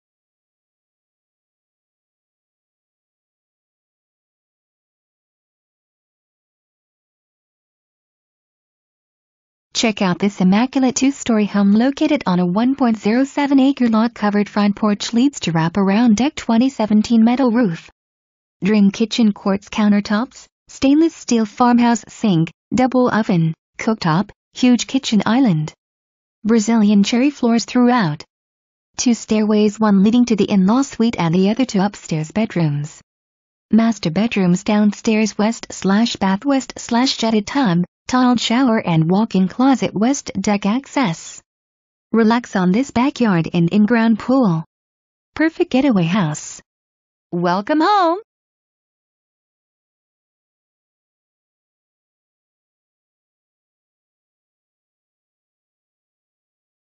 419 Senter Ln
Check out this Immaculate Two-Story Home located on a 1.07 Acre Lot Covered Front Porch Leads to Wraparound Deck.2017 Metal Roof!DREAM KITCHEN-Quartz Countertops,SS Farmhouse Sink,Double Oven,Cooktop, HUGE Kitchen Island. Brazilian Cherry Floors Throughout! Two Stairways-One Leading to the In-Law Suite & The Other to Upstairs Bedrooms. Master Bedrooms Downstairs w/ Bath w/ Jetted Tub,Tiled Shower & WIC w/Deck Access! Relax on this Backyard & inground pool. Perfect getaway house! Welcome Home!

Presented By:
Sharon Hedgepeth, Keller Williams